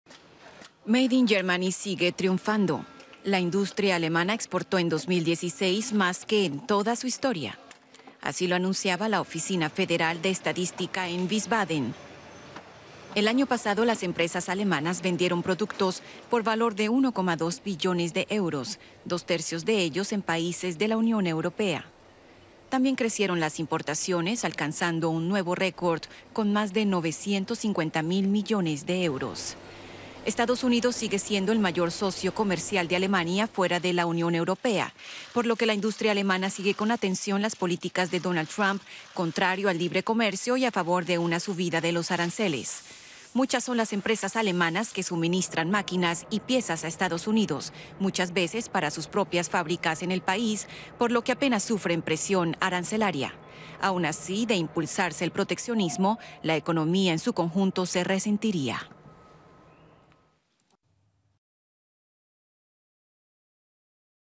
Rekord für "Made in Germany"
Aun así se teme un deterioro del comercio por las pretensiones proteccionistas de Donald Trump.